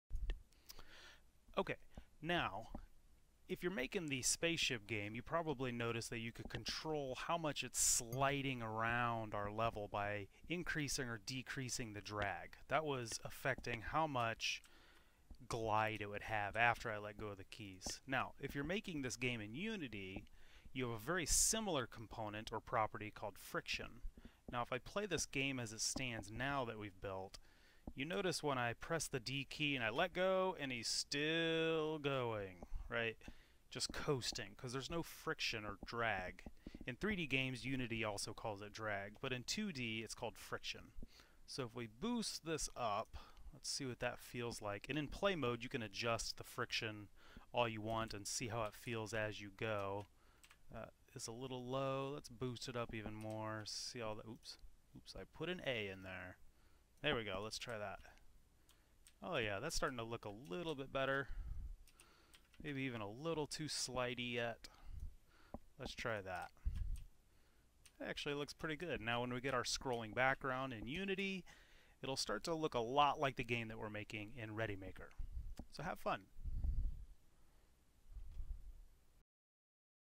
Ready Maker to Unity
This is a demo clip as part of a larger project to transition users from Ready Maker's drag n drop to Unity 2D Game development for beginners.